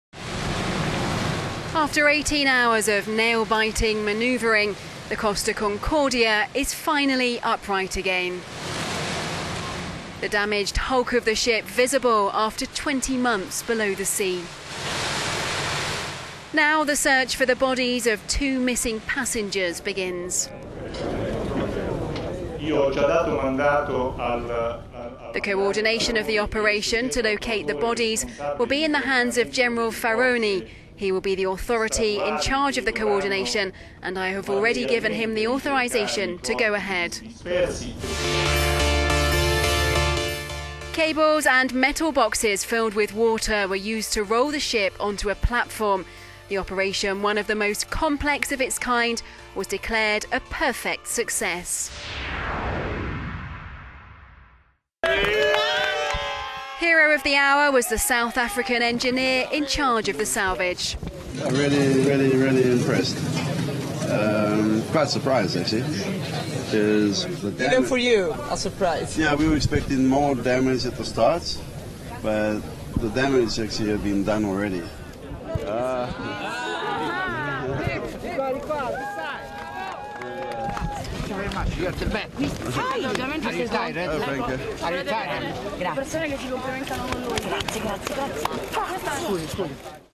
Italy: search for missing Costa Concordia passengers to begin
The hunt for the bodies of two missing passengers who died when the Costa Concordia capsized is to...

The hunt for the bodies of two missing passengers who died when the Costa Concordia capsized is to begin, now that the cruise ship has been successfully salvaged.

In a complex 18 hour operation, engineers used cables and metal boxes filled with water to roll the ship onto a platform. The liner will now be reinforced, and towed away in the spring.

The authorities did warn it could take days before they could start looking for the bodies. "From an operative point of view we should wait until the shipwreck is secured," said civil protection official Franco Gabrielli.

Nick Sloane, the South African chief salvage master was congratulated on the successful recovery operation. He said he was pleased it had gone to plan: "I am really, really impressed. Quite surprised actually. We were expecting more damage at the start. But the damage had actually been done already."

The Costa Concordia ran aground near the Italian island of Giglio in January 2012, killing 32 people.

==Costa Concordia: A 'perfect operation'Source: Anadolu press agency==

==Tweets por @InfoParbuckling==